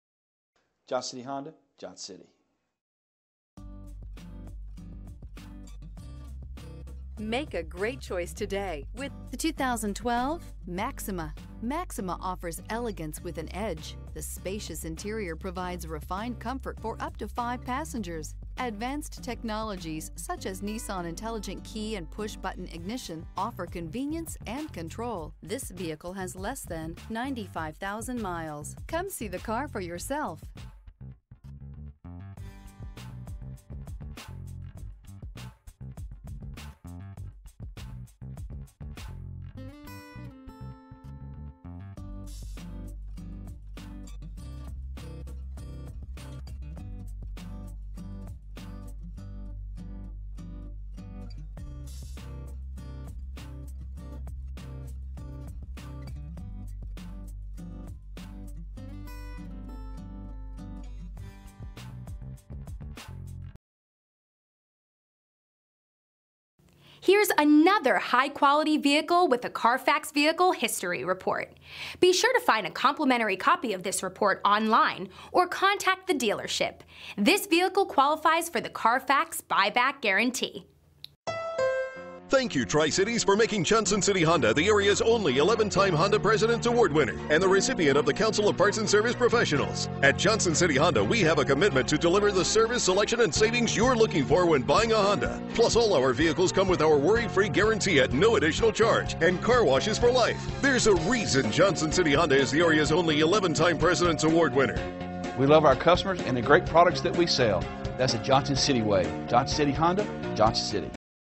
2012 Nissan Maxima 3.5 SV Used 230324A
2012 Nissan Maxima 3.5 SV

Pick up this  2012 Nissan Maxima, available today at Johnson City Honda. This could be the one you've been searching for.